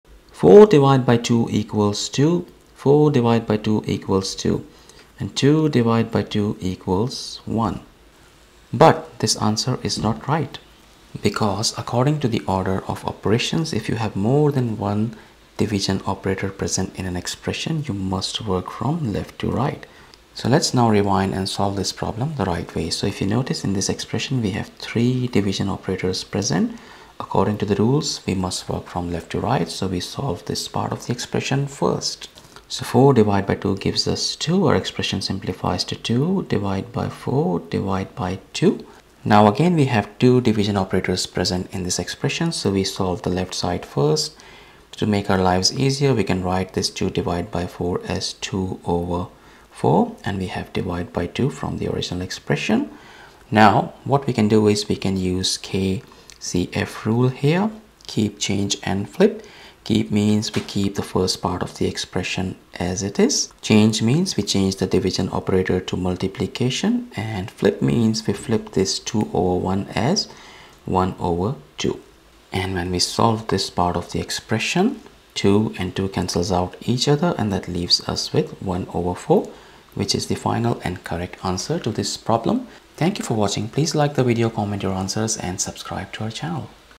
Only 5% Can Solve This Viral Math Problem Super Fast!
What is the answer to 4 ÷ 2 ÷ 4 ÷ 2? Let's rewind and solve it the right way! This viral math problem is stumping the internet, but the solution is clear once you know the rules. 🔥

In this video, I'll break down the Order of Operations (PEMDAS/BODMAS) step-by-step:
• Parentheses First: Where everyone goes wrong
• Exponents & Operations: The correct sequence
• Final Answer: The mathematically correct solution

📚 What You'll Learn:
• How to confidently solve any viral math problem
• The foolproof PEMDAS rules that never fail
• Why implied multiplication causes so much debate
• How to avoid common calculation traps

👉 Ready to master basic math? Watch to the end to never get tricked again!

Enjoyed this?
✅ LIKE if you got the right answer!
✅ COMMENT with your answer before watching - did you get it right?